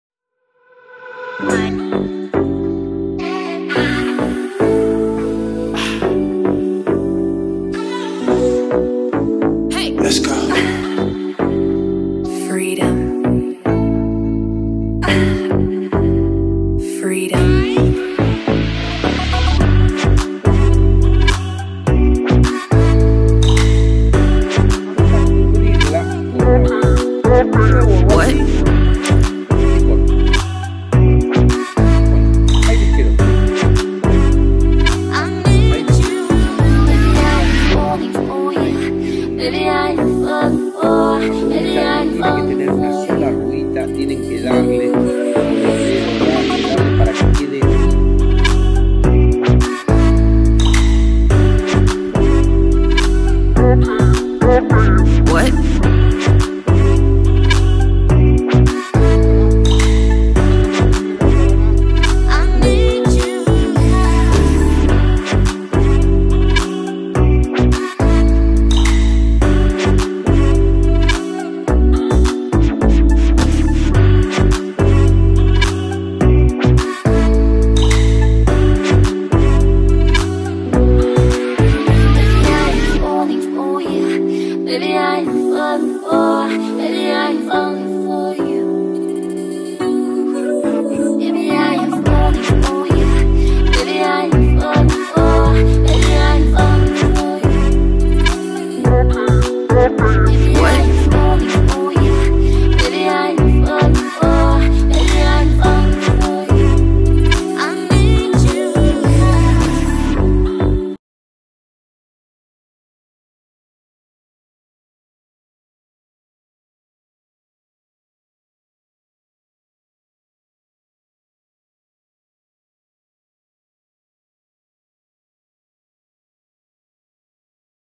CONTESTAMOS PREGUNTAS Y DAMOS INFORMES.
TODAS LAS PREGUNTAS SOBRE LA PESCA DEPORTIVA LAS DESPEJAMOS EN EL VIVO .